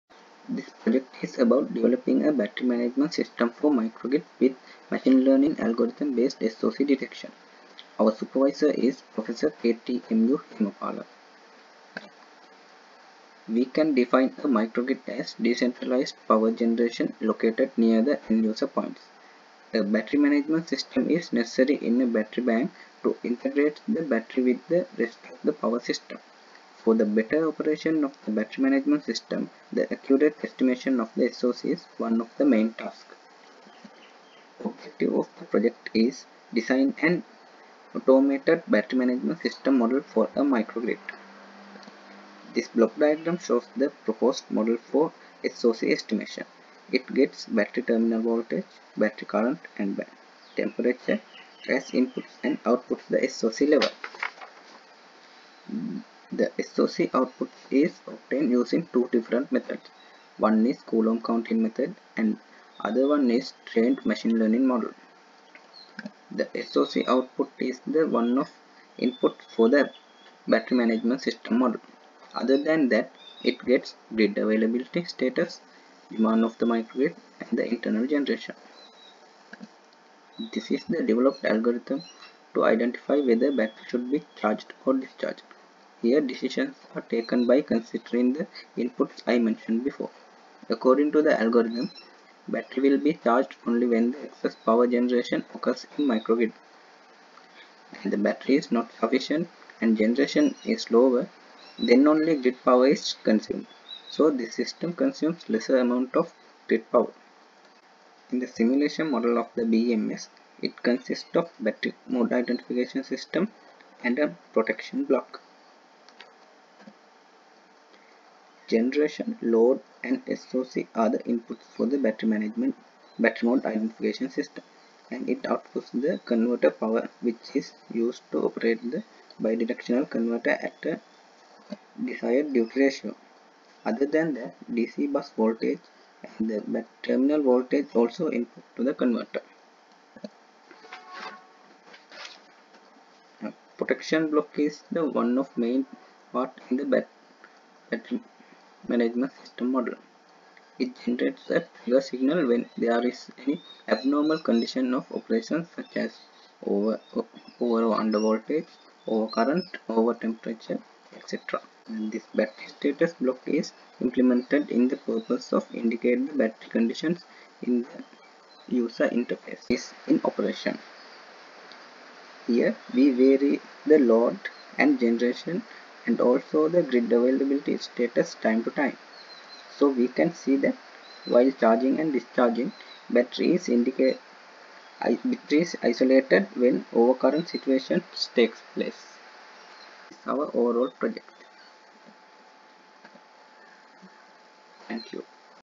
Developing a Battery Management System for Microgrids with Machine Learning
Group Memebers:
                              M N M Nifran
                              D P S Bandara
                              S P A P B Sudasinghe

Supervisor:         Prof. K T M U Hemapala